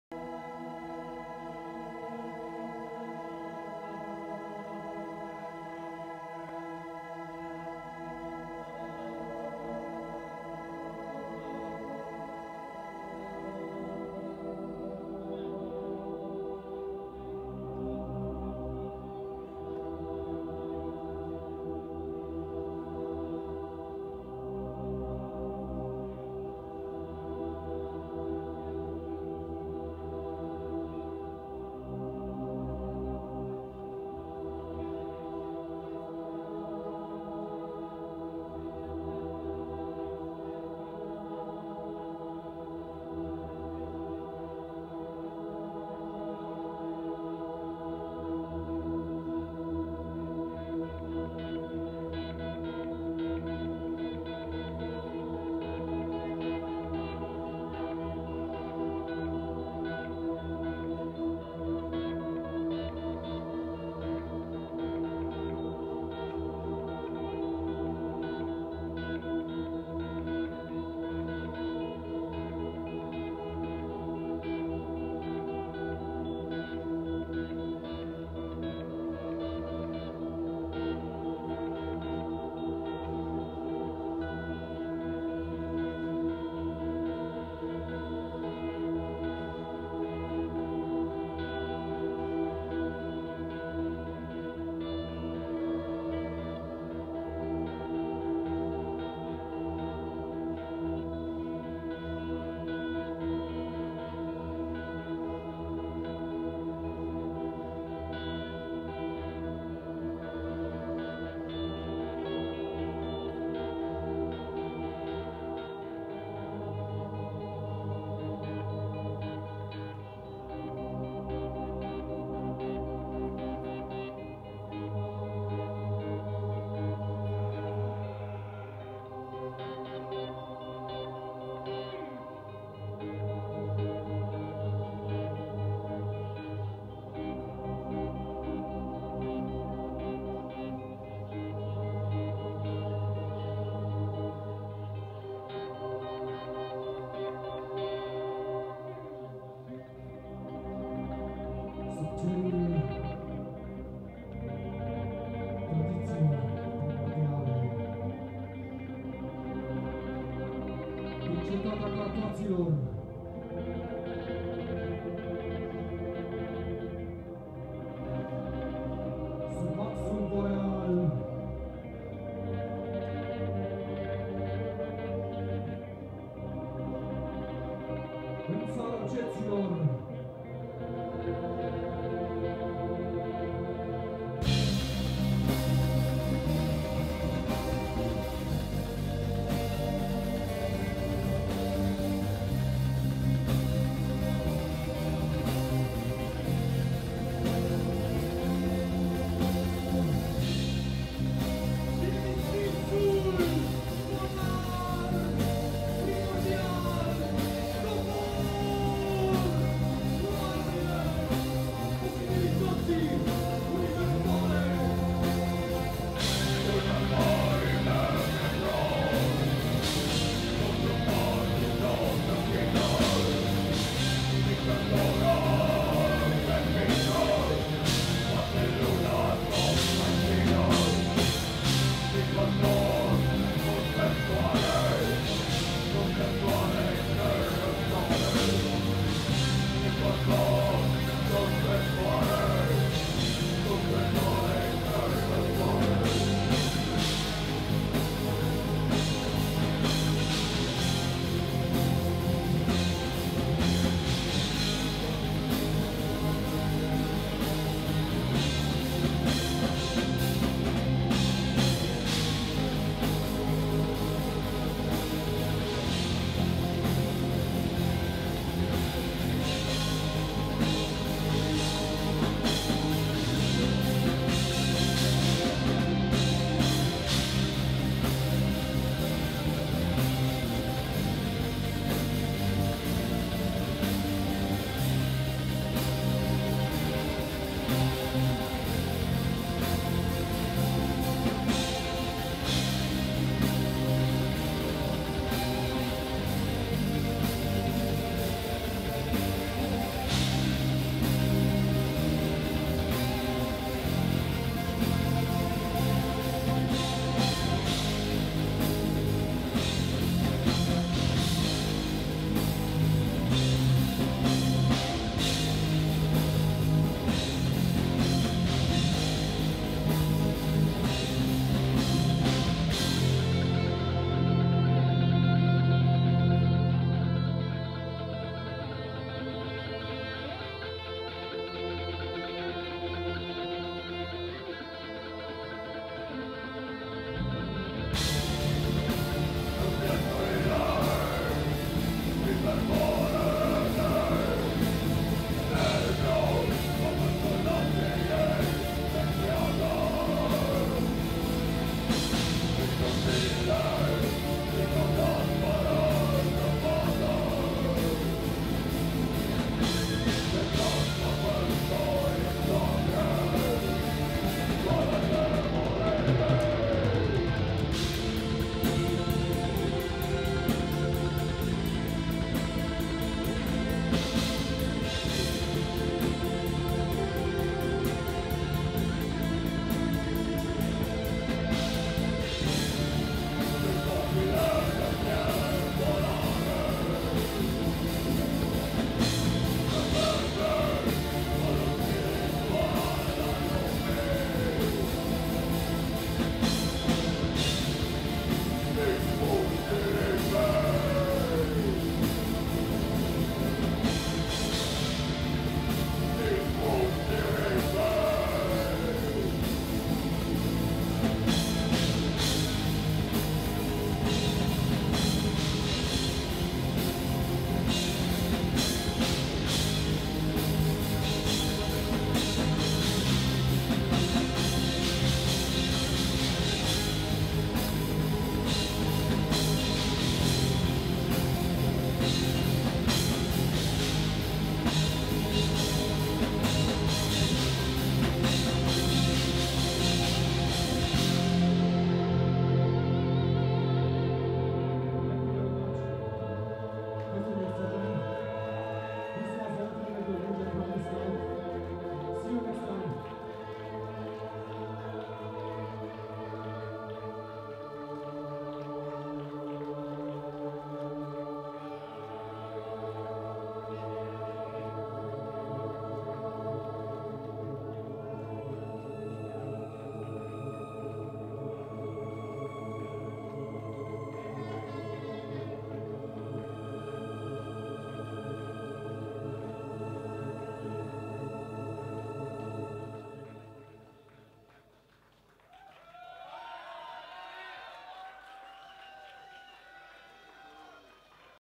Negura Bunget (Live at Fekete Zaj fest 2011)
Negura Bunget performing their last song (of their very short set) at Fekete Zaj festival, 5 August 2011.
Messed up the sound a little, sorry.